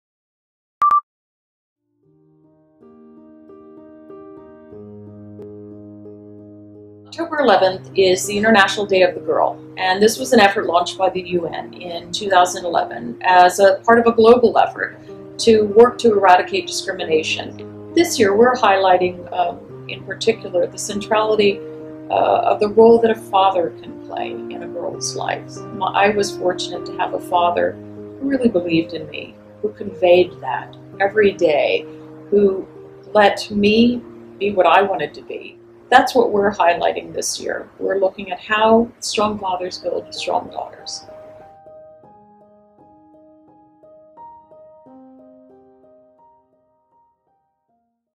US Ambassador Barbara Leaf Explains Dads and Daughters Day
What is the International Day of the Girl and why does this year have a focus on “Dads and Daughters”? Watch US Ambassador Barbara Leaf explain in a personal message.